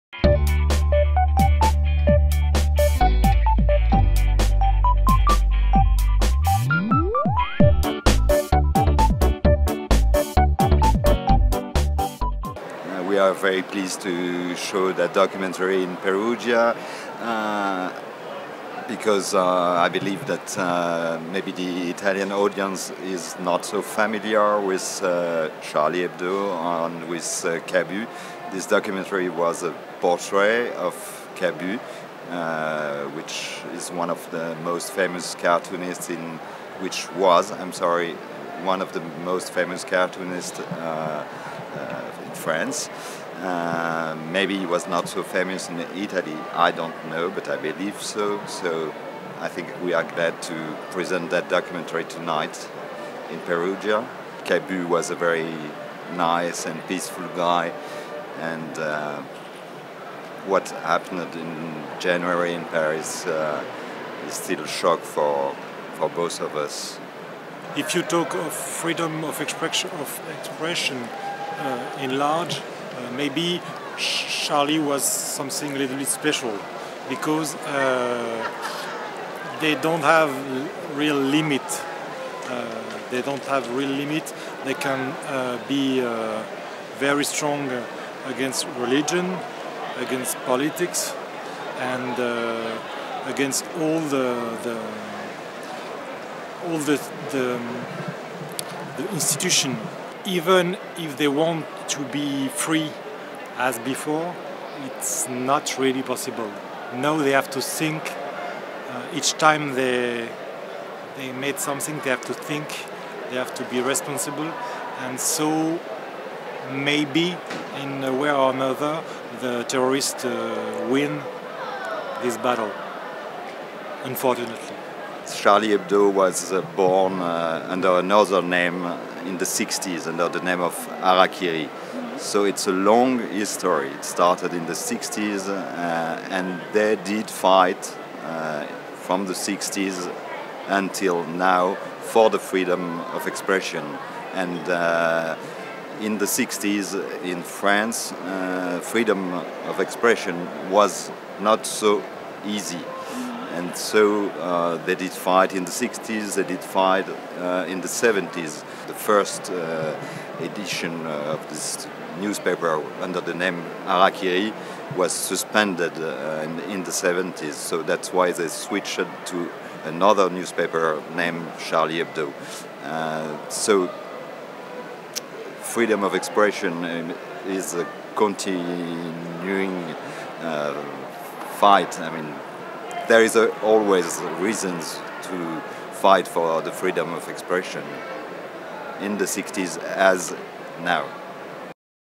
Web Magazine - Cabu, politiquement incorrect!
IIn memory of the victims of the attack on Charlie Hebdo
Interview by Sofia Lotto Persio
Shooting by di Lisa Miraldi
Editing by Giulia Perona